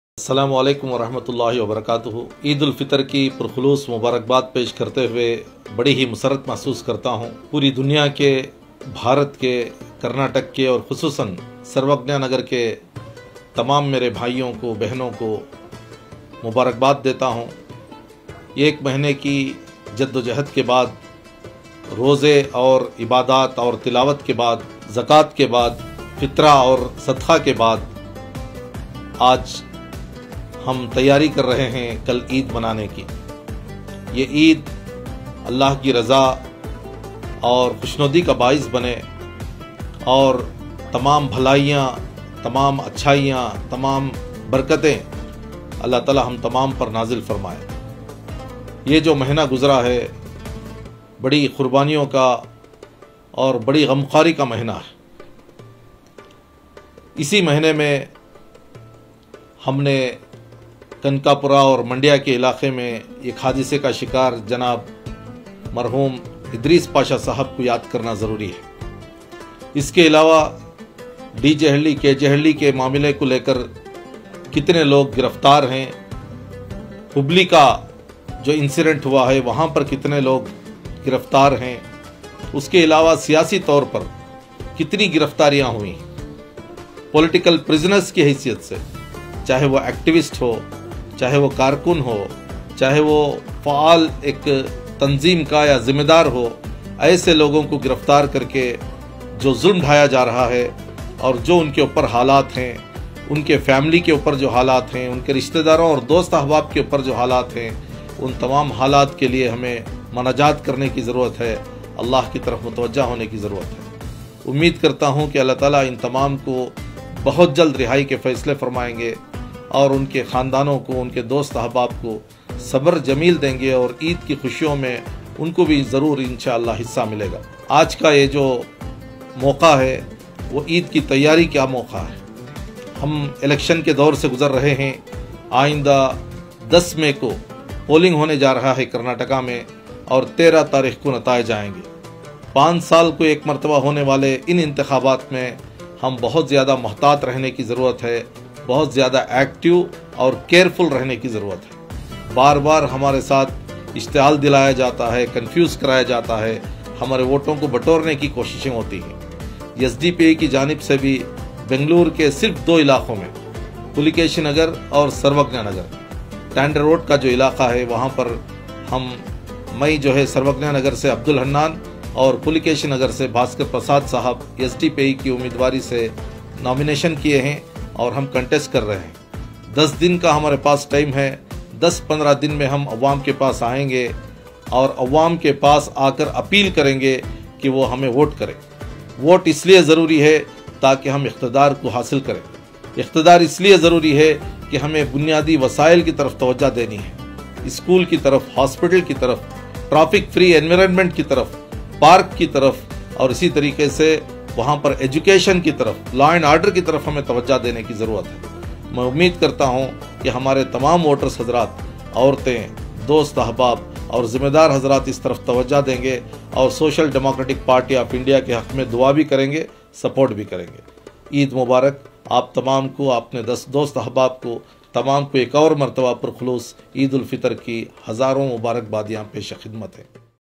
Eid Message | Marhoom Idrees Pasha ko aur Tamaam Political Prisoners ko apni dua'on me yaad rakhein
Eid Message | Marhoom Idrees Pasha ko aur Tamaam Political Prisoners ko apni dua'on me yaad rakhein - Abdul Hannan, MLA Candidate, Sarvagnanagar Assembly Constituency